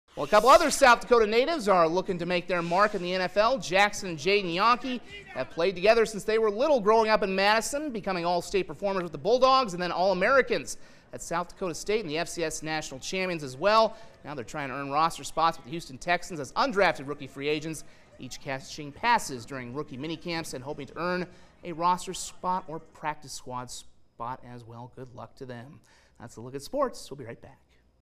Madison's Janke brothers look to catch on with Houston Texans
SDSU alums sign on as undrafted rookie free agents